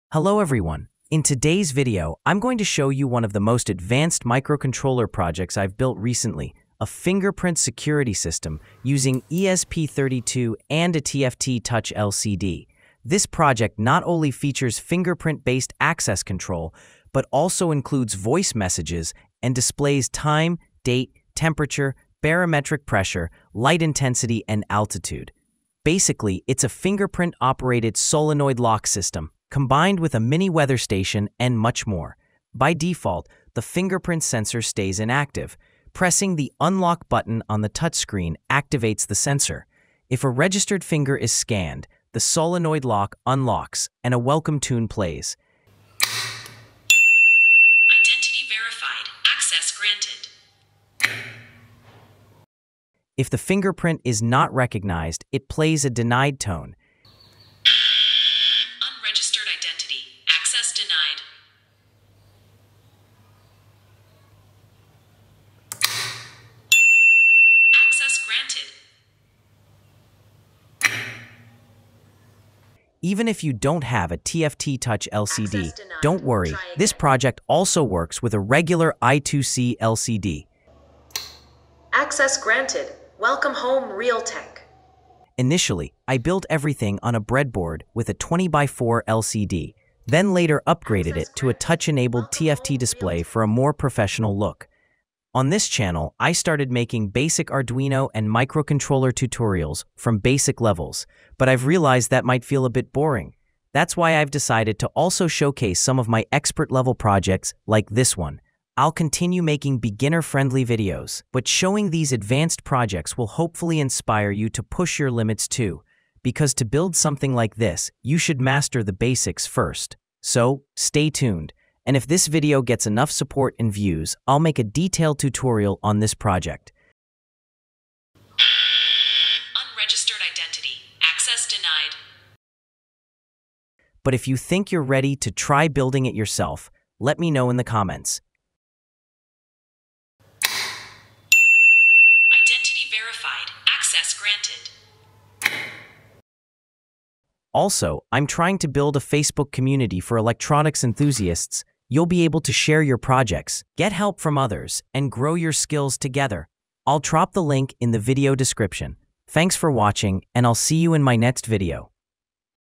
I Built a Fingerprint Security System with ESP32 + TFT LCD and It Works
In this video, I’m showcasing one of my most advanced microcontroller projects: a Fingerprint-based Solenoid Lock System powered by the ESP32 and a TFT Touch Display. But it’s more than just a door lock — this smart security system includes:

✅ Fingerprint authentication
✅ Voice messages and welcome tune
✅ Real-time clock & date display
✅ Temperature, pressure, light intensity, and altitude monitoring
✅ Dual display support: TFT LCD & I2C 20x4 LCD

Whether you're a beginner or an advanced builder, this project brings together multiple modules in a clean and efficient way.

Modules and Components Used:

1. ESP32 NodeMCU Development Board
2. R307S Fingerprint Sensor
3. TFT RGB LCD (2.8-inch Touch)
4. DFPlayer Mini + 3W Speaker (for audio)
5. BMP280 Barometric Pressure Sensor
6. BH1750 Light Sensor
7. RTC Module
8. RGB LED Module
8. Solenoid Lock + Relay Module
9. 2N2222 Transistor + 1K Resistor

Join our growing electronics community on Facebook to share and get help.